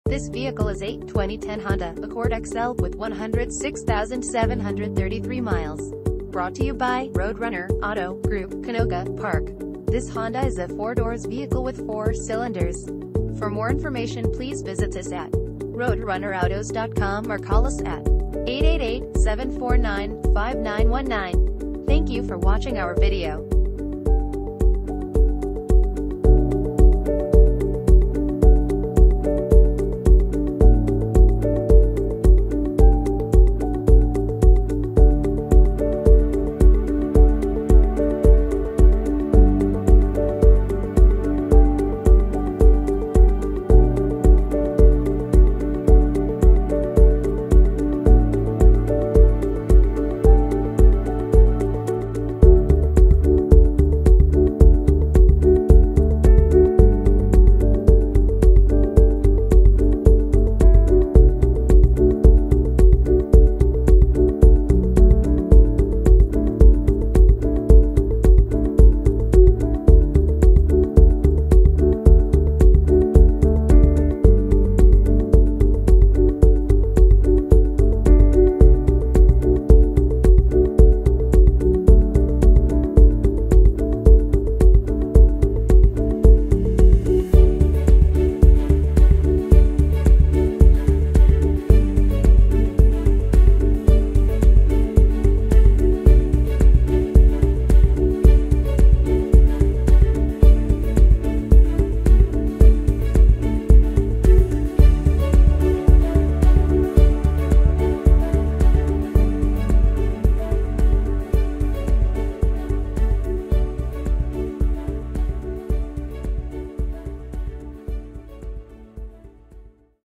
2010 Honda Accord EX-L - Roadrunner Auto Group - Canoga Park
2 owners, Clean title, Extended Warranty Available, 106,733 miles, Automatic, Polished Metal Metallic exterior, Gray interior, Sedan, FWD: Front Wheel Drive, 4 doors, 4 cylinders, Gasoline, MPG City 21 and 31 in Hwy.